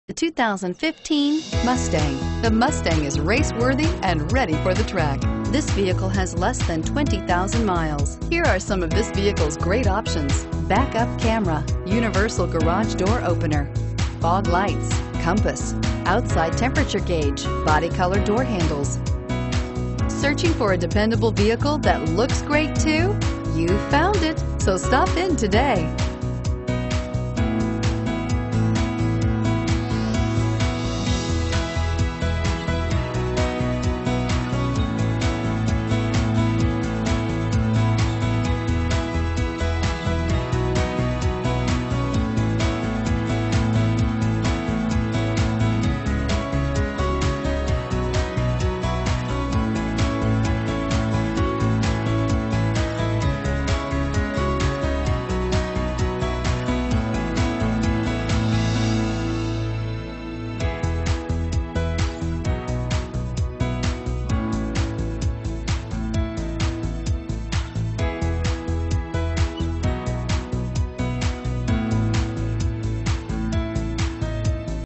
2015 Ford Mustang Terrell TX D5441A
2015 Ford Mustang

We are pleased to present this excellent 2015 Ford Mustang with 16000 miles. Its beautiful Competition Orange exterior is nicely complimented by a clean Ceramic interior.  This Mustang comes equipped with the following options,  ENGINE: 2.3L ECOBOOST, Black Grille, Black Side Windows Trim, Body-Colored Door Handles, Body-Colored Front Bumper, Body-Colored Power Heated Side Mirrors w/Convex Spotter,  Manual Folding and Turn Signal Indicator, Body-Colored Rear Bumper, Clearcoat Paint, Fixed Rear Window w/Defroster,  and much more. For a complete list, select the Options button below.